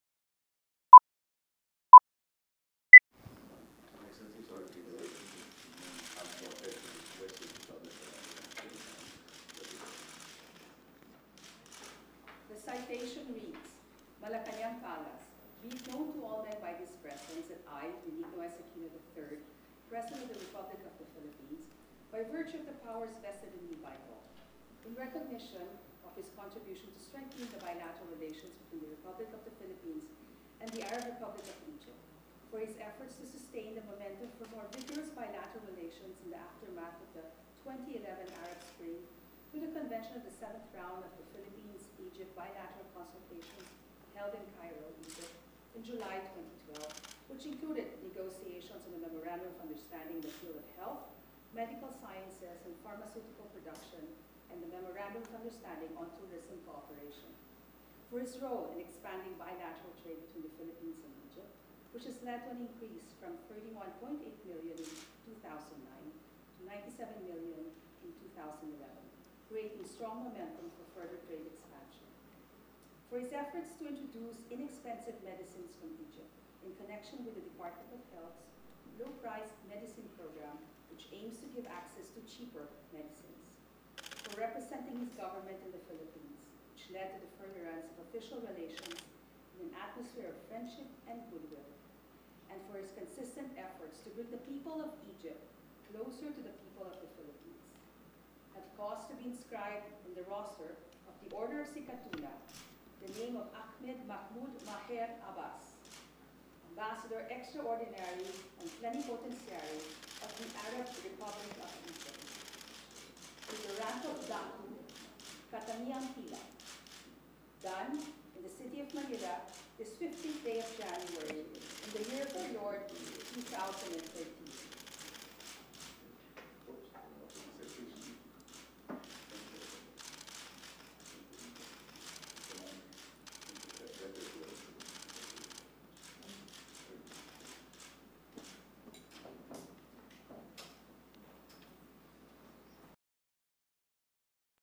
Farewell Call of Egyptian Ambassador Ahmed Mahmoud Maher Abbas 01/15/2013
Malacañan Palace
15 January 2013

President Benigno S. Aquino III conferred the Order of Sikatuna with the Rank of Datu, Katangiang Pilak, to outgoing Ambassador Ahmed Mahmoud Maher Abbas of Egypt in a farewell call held in Malacañan Palace.

The Egyptian Ambassador received the Grand Cross (Silver Distinction) from the President as recognition for the exceptional services he rendered to the Philippines and for strengthening the relations between the two countries.

Ambassador Abbas has been further commended for his efforts in sustaining bilateral relations in the aftermath of the 2011 Arab Spring which included a Memorandum of Understanding in the field of medical sciences, pharmaceutical production and tourism cooperation. During his term, bilateral trade between the two countries had increased from $31.8 million in 2009 to $97 million in 2011.

Additionally, Ambassador Abbas also introduced inexpensive medicines from Egypt, partnering with the Department of Health's Low Price Medicine Program which aims to provide cheaper medicines.

Undersecretary Jose Brilliantes, Acting Secretary for the Department of Foreign Affairs (DFA) also attended the ceremony.